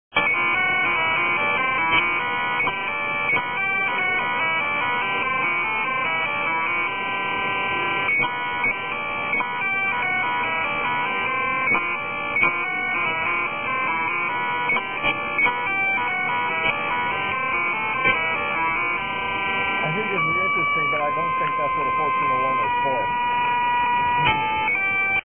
1401-music-Movie
A tune played on the 1401 and heard through a hand-held AM radio held next to it.